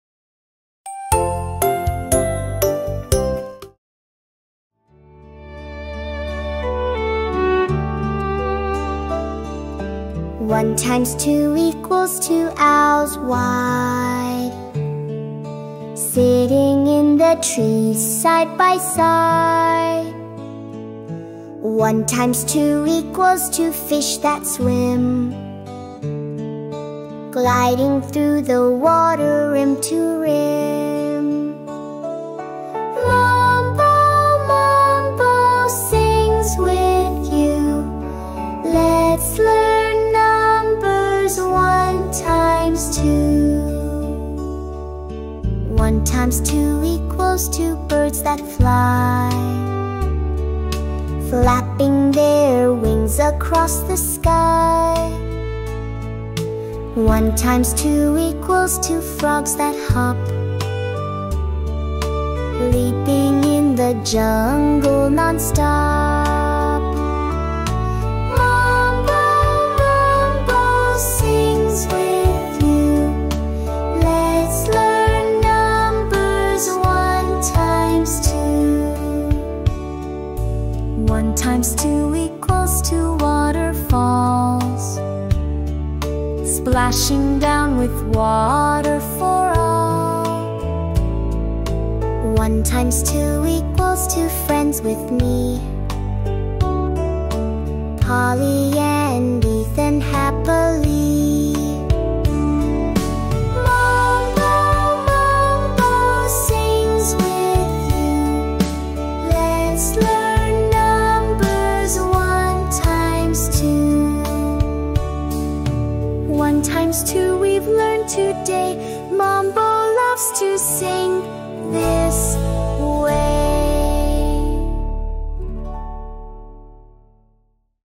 Mombo Solves 1 x 2 | Education with Mombo
All About Multiplying 1 × 2 is a playful and musical journey through the jungle with Mombo! Kids will discover how numbers come together when we multiply — all while singing, learning, and exploring. A perfect early education video for toddlers and preschoolers!